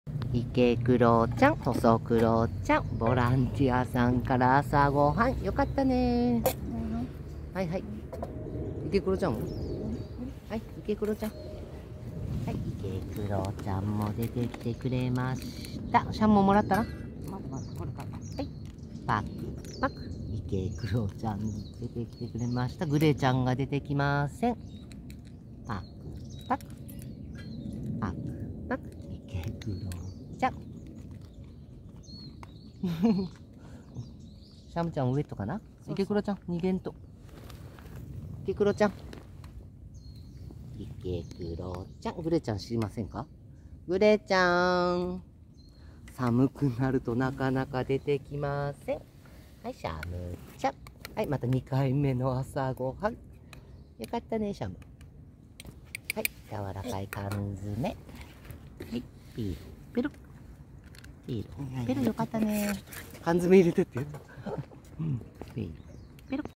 池黒ちゃんも出て来てくれました❗ボランティアさんから朝ごはん❗良かったね❗グレーちゃんは寝坊かニャ～💤❔❤️
池黒ちゃんも出て来てくれました❗
ボランティアさんから朝ごはん❗
良かったね❗
グレーちゃんは寝坊かニャ～💤❔❤️

Ikeguro-chan also came out❗
Breakfast from the volunteers❗
That's great❗
Did Gray-chan oversleep? Meow~💤❔❤️